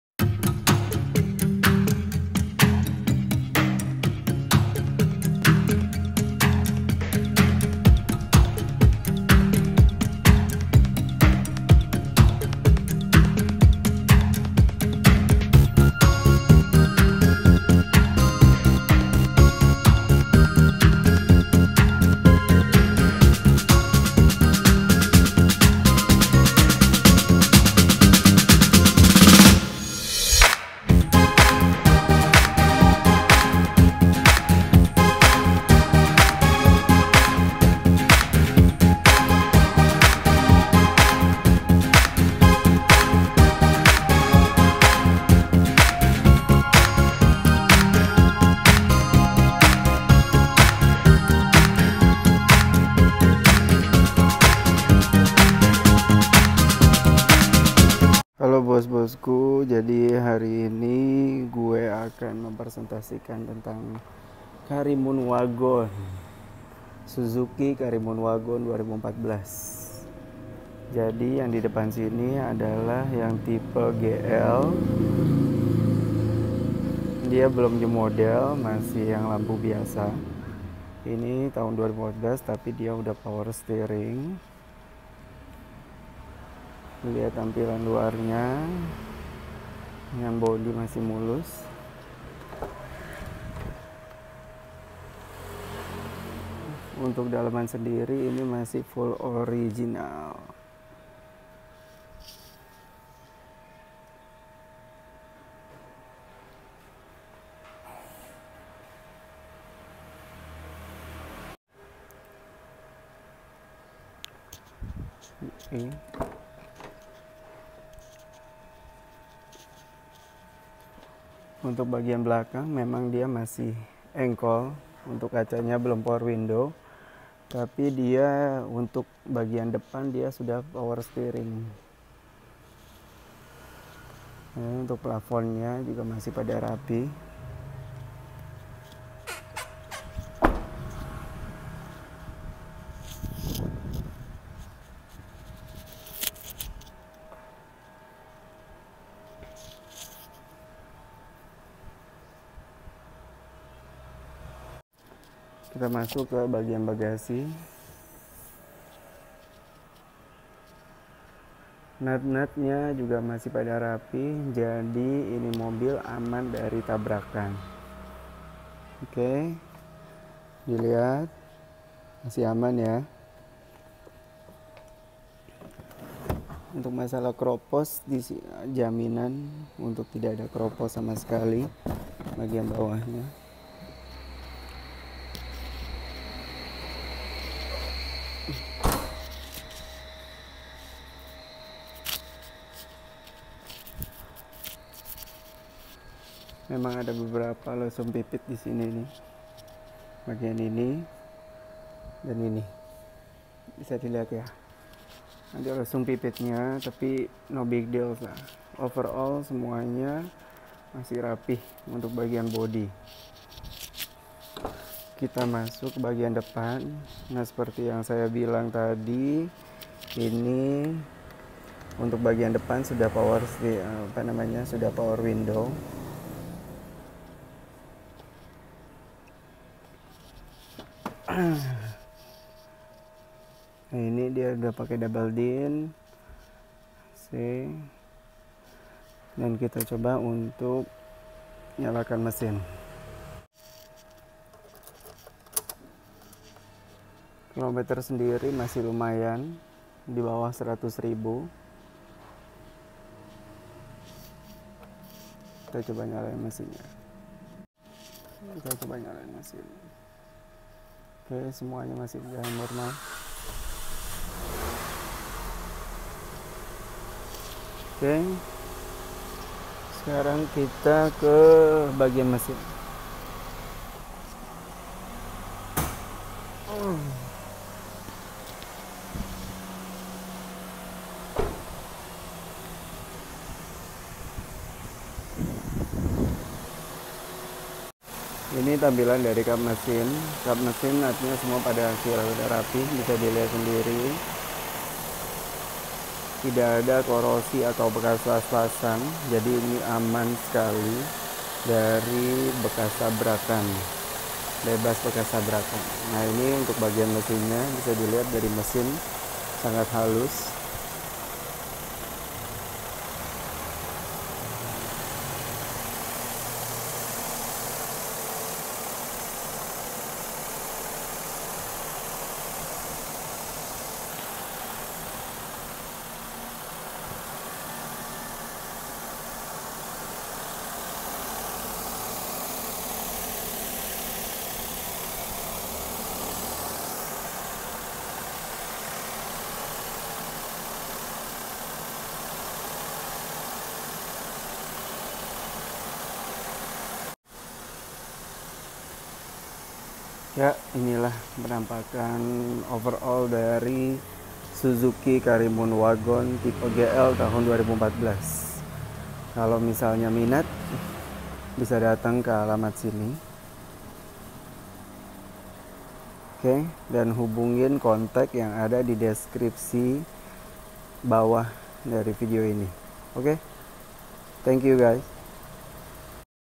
Suzuki Karimun Wagon R 2014
For Sale !!

Tipe GL
With Velg Racing and Power Window
Good condition